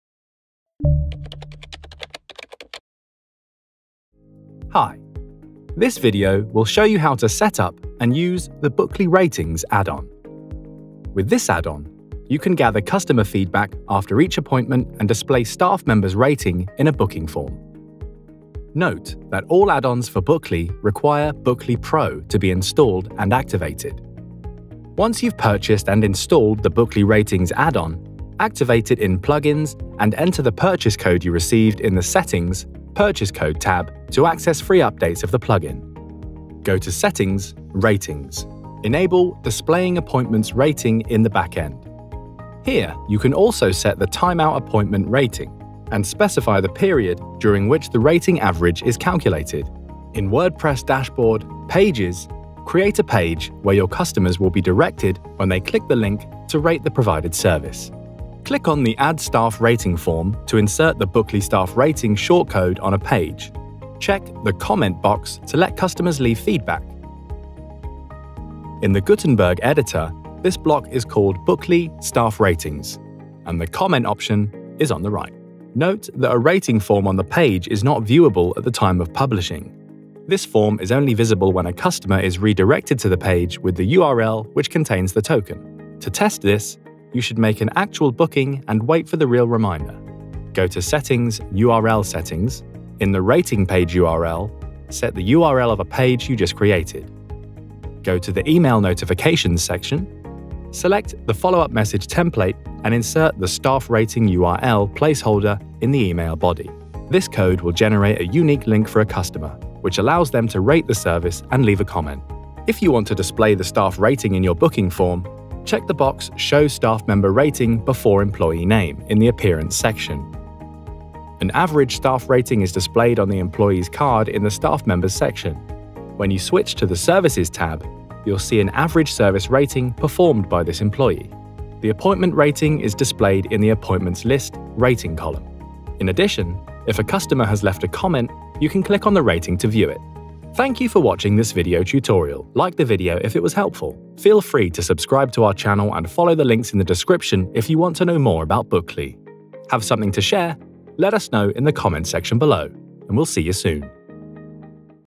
How to let clients rate your services – Bookly Ratings (Add-on)
This video will show you how to set up and use the Bookly Ratings (Add-on). With this add-on, you can gather customer feedback after each appointment and display staff members’ rating in a booking form.

Check the links below, and be the first to know about Bookly news, discounts and more!